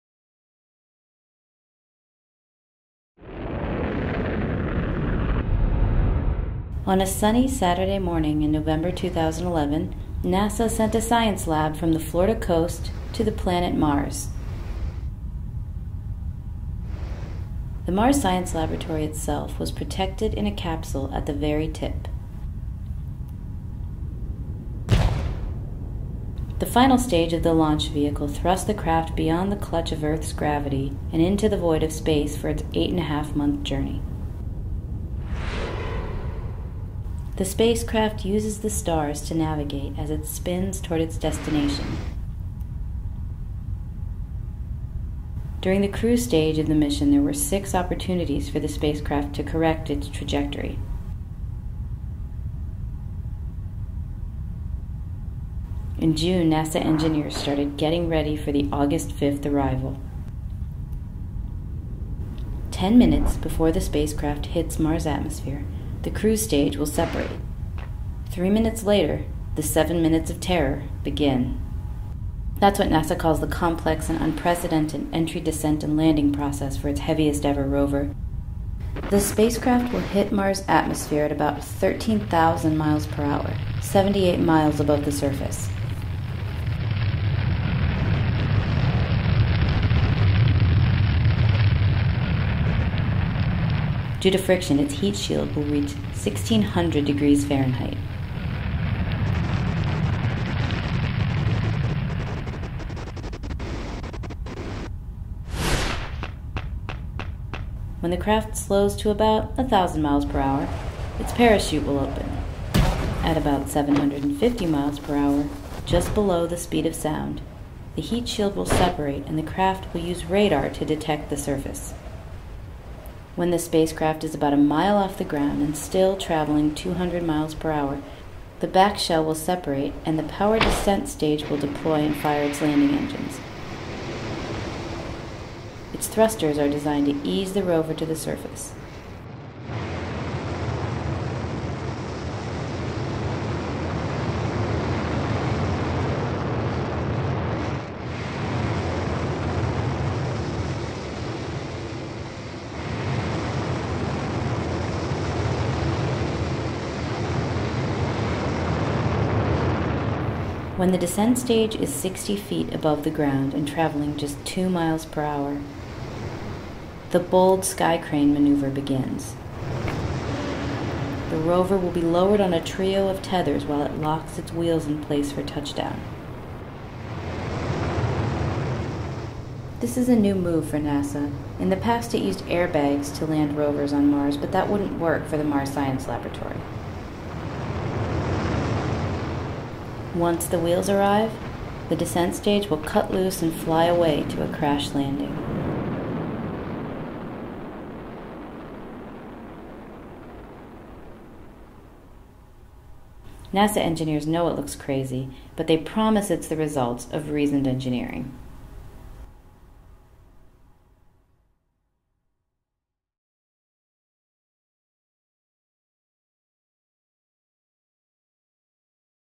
Mars Science Lab: 7 minutes of terror
This video provides a quick overview of the Mars Science Laboratory mission, including the "7 minutes of terror" that NASA coined to describe the unprecedented landing plan designed for the mission.  Michigan Engineering faculty, staff and students have played a role in the NASA mission, which will set the Curiosity rover on a carefully selected site called Gale Crater. Learn more about the Mars Science Laboratory and U-M involvement in this Digital Multimedia Experience

ABOUT THE PEOPLE: U-M people involved in the mission include:
- Mark Pokora, AERO grad
- Manish Mehta, AOSS grad
- Nilton Renno, AOSS professor
- Leslie Hall, AOSS professor
- Sushil Atreya, AOSS professor